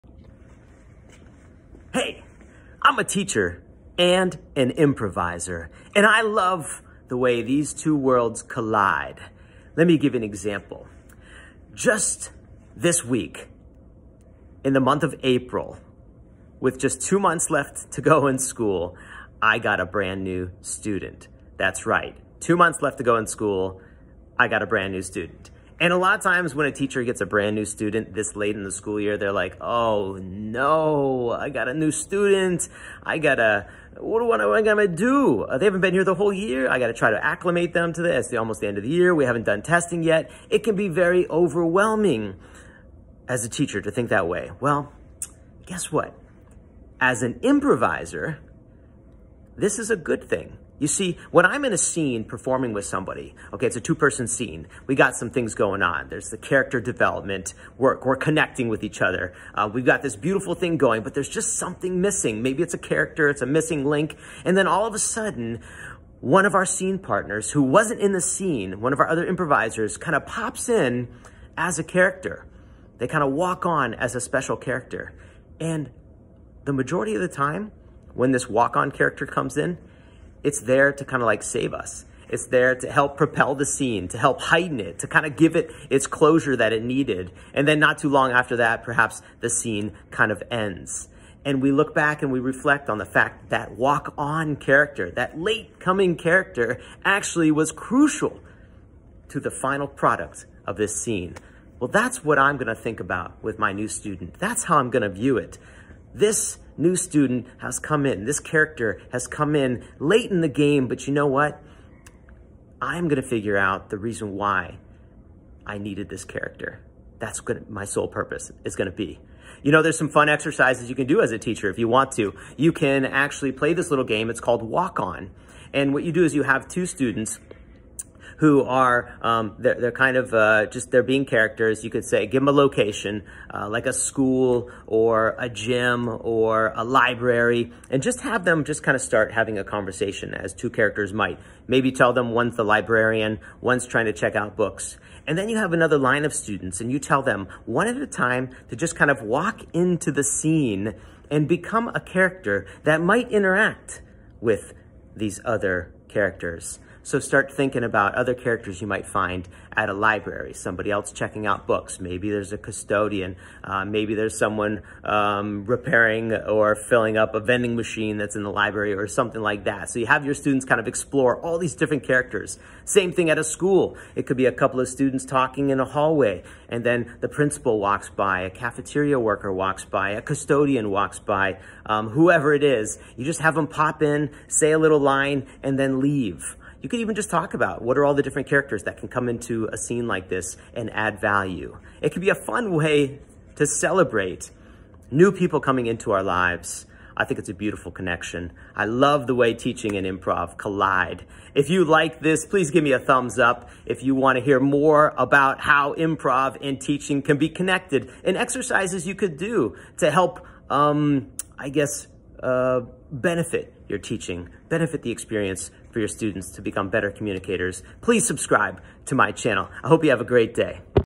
New Student No Problem
In improv and in life, we are often introduced to new  characters/people. There are ways you can train for when this happens so you are prepared and can recognize the benefits.